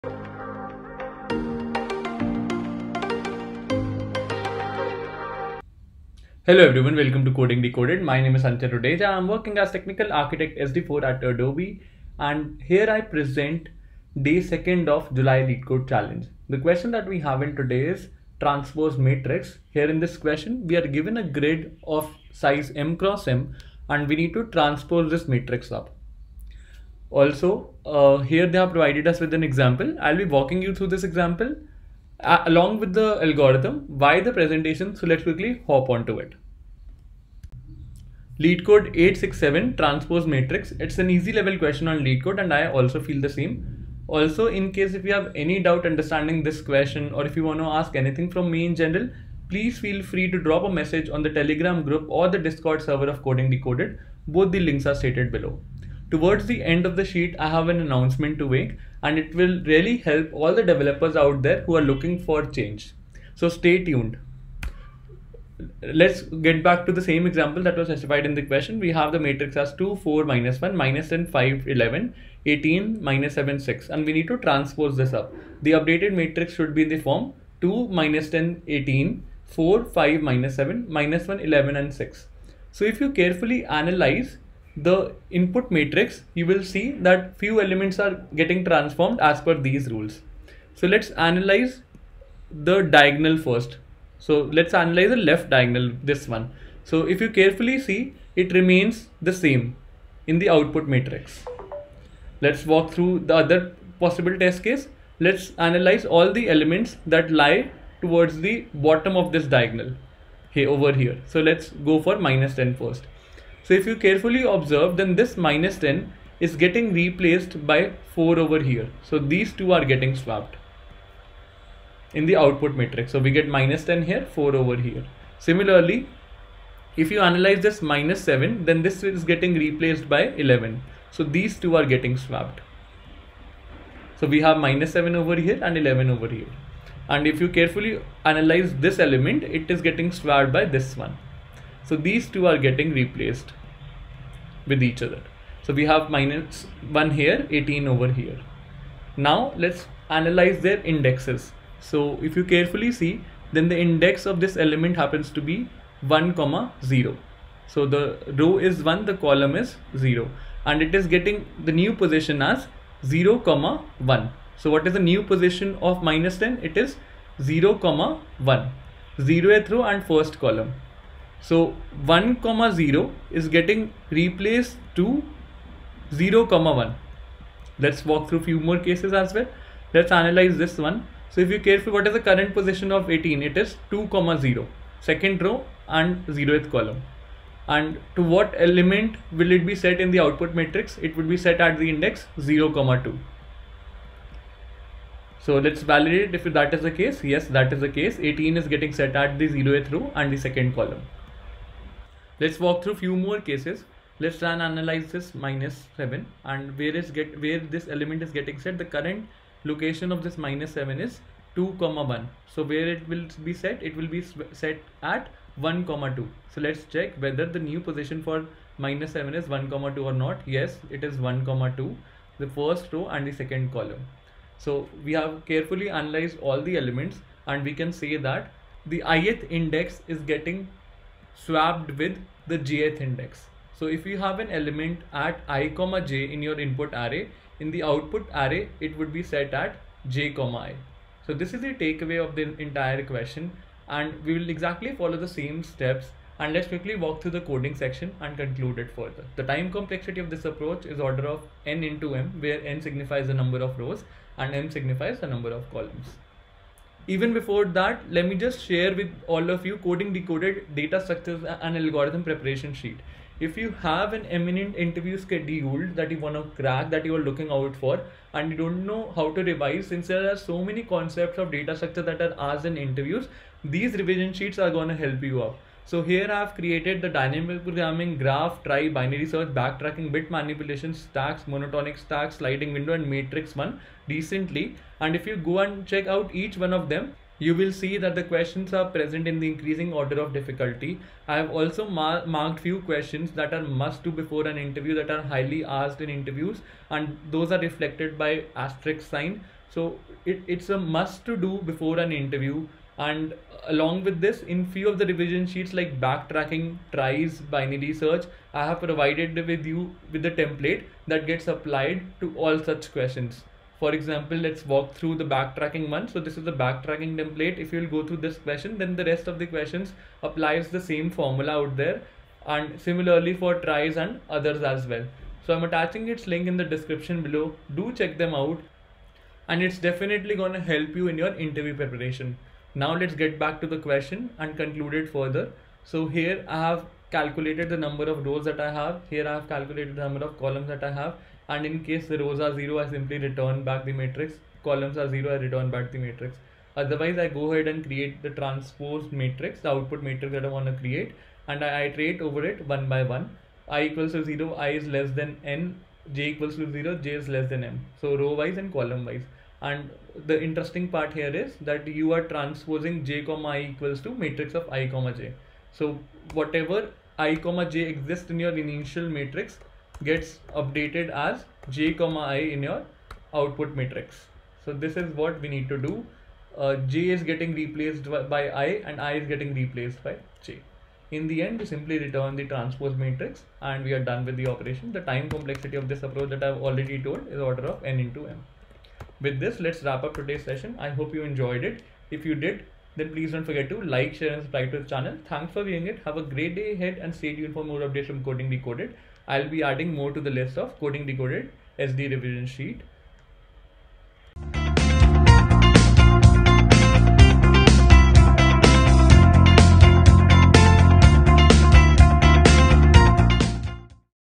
Transpose Matrix | Leetcode 867 | Live coding session 🔥🔥
Here is the solution to "Transpose Matrix"  leetcode question. Hope you have a great time going through it.

🎉Chapters
0:00 Intro
1:30 Understand the algorithm
4:00 Coding Decoded SDE Revision Sheet
5:00 Coding it up